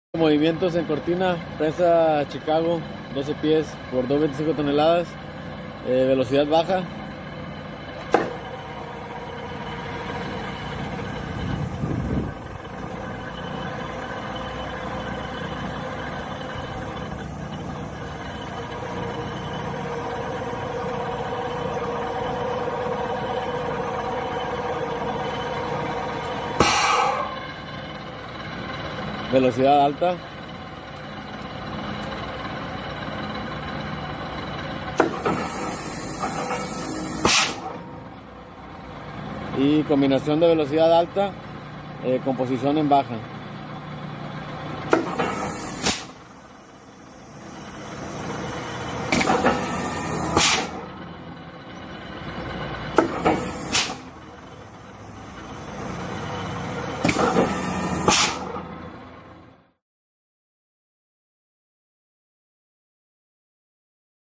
Prensa CHICAGO 12' x 225 Ton
DATOS TÉCNICOS
CHICAGO

Prensa
Capacidad:12' x 225 ton
Distancia entre paredes: 10’ 6”
Velocidad: alta y baja
Velocidad combinada
Cama: 12’ 6”
Año: 1973
Peso: 37,000lbs
Modelo: 225D-10